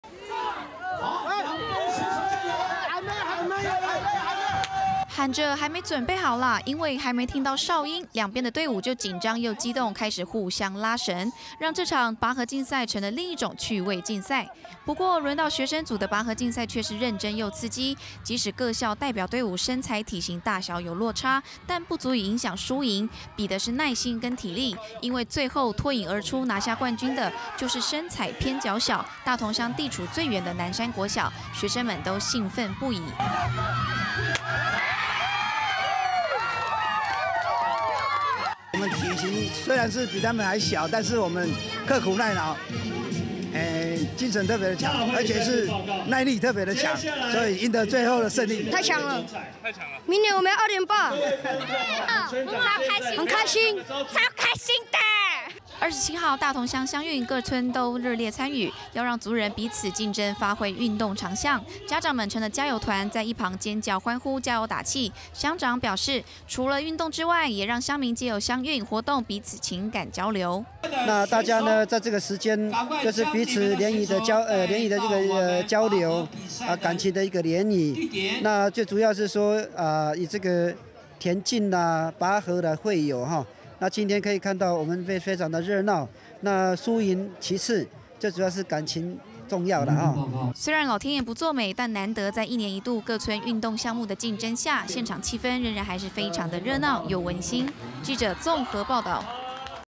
大同鄉運雨中登場 鄉民熱情參與 20130427
【綜合報導】

喊著還沒有準備好啦，因為還沒聽到哨音，兩邊的隊伍就緊張又激動開始互相拉繩，讓這場拔河競賽成了另一種趣味競賽，不過輪到學生組的拔河競賽卻是認真又刺激，即使各校代表隊伍身材體型大小有落差，但不足以影響輸贏，比的是耐心跟體力，因為最後脫穎而出拿下冠軍的就是身材偏矯小、大同鄉地處最遠的南山國小，學生們都興奮不已。

4/27大同鄉鄉運各村都熱烈參與，要讓族人彼此競爭發揮運動長項，家長們成了加油團在一旁尖叫歡呼、加油打氣，鄉長表示除運動外，也讓鄉民藉由鄉運活動彼此情感交流。

雖然老天爺不作美，但難得在一年一度各村運動項目的競爭下，現場氣氛仍然還是非常的熱鬧又溫馨。